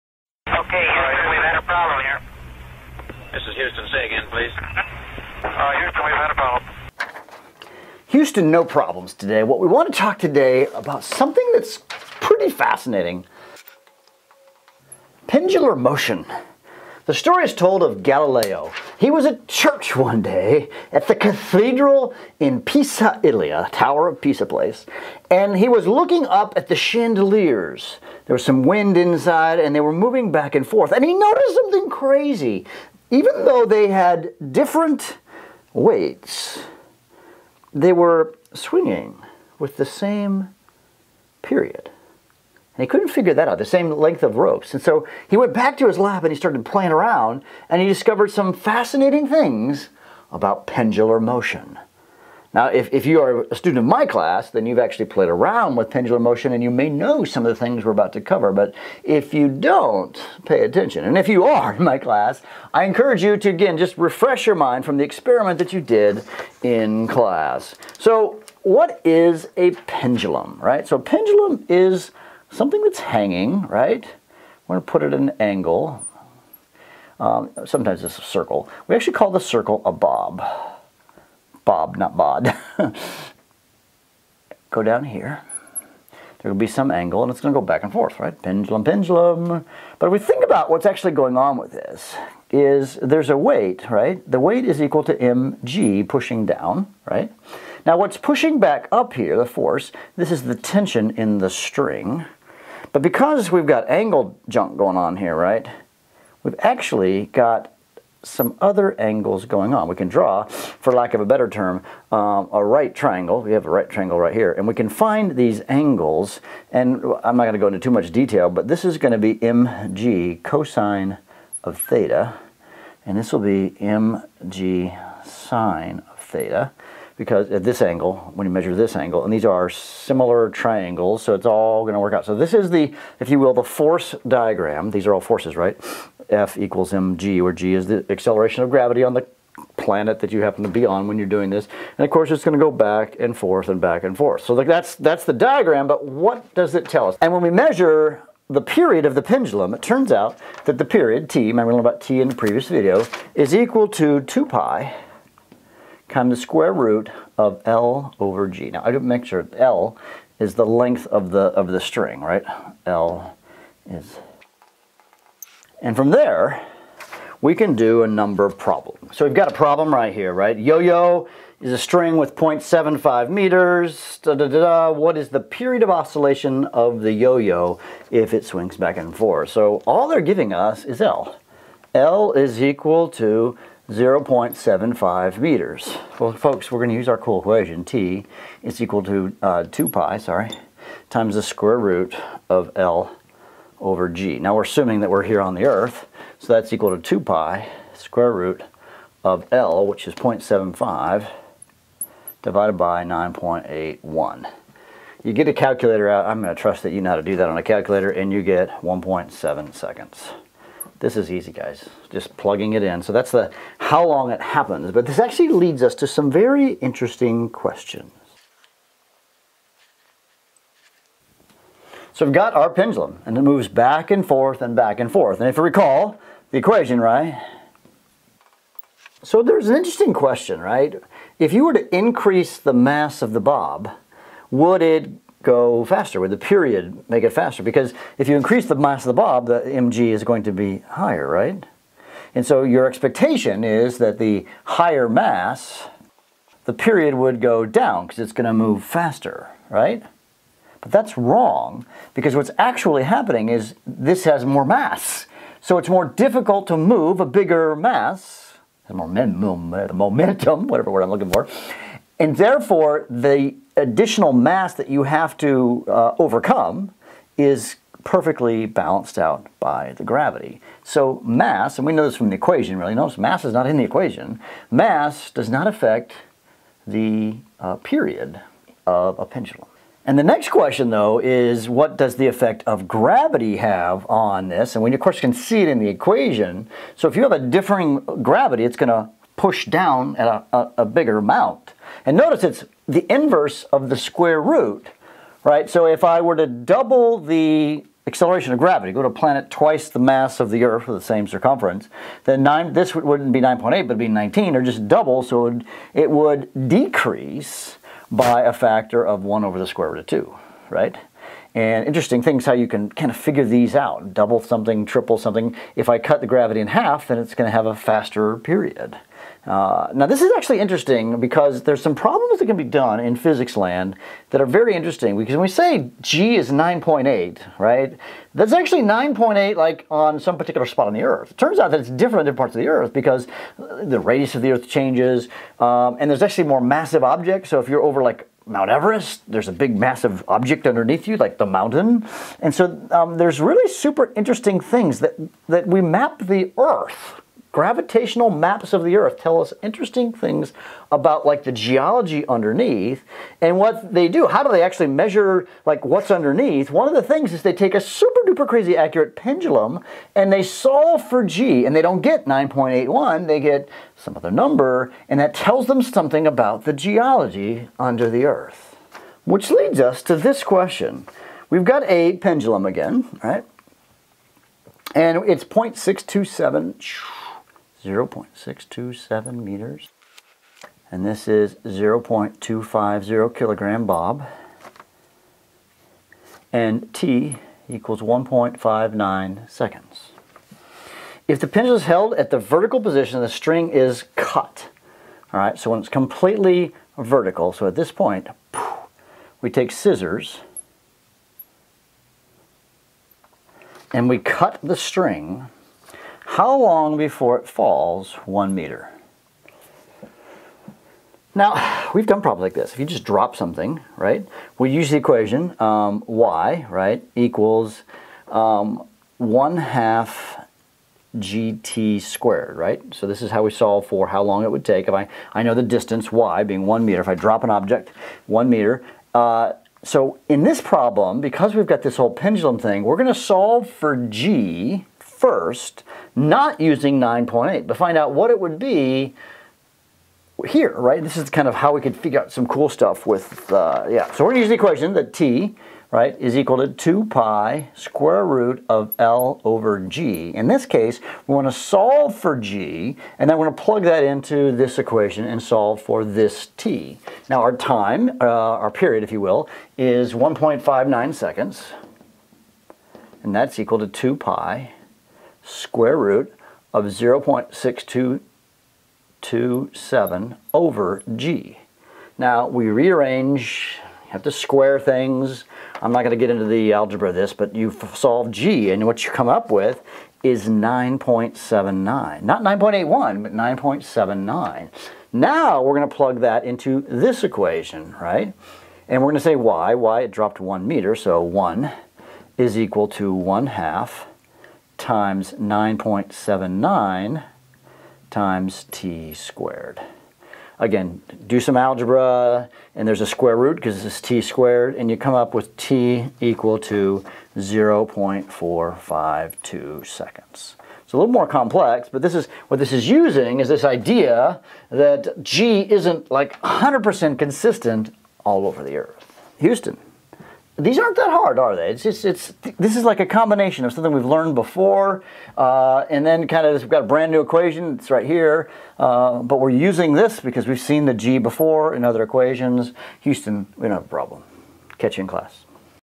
Pendular Motion Simplified
How do pendulums work? What does their motion depend on?  Length of the string? Mass of the bob? Something else?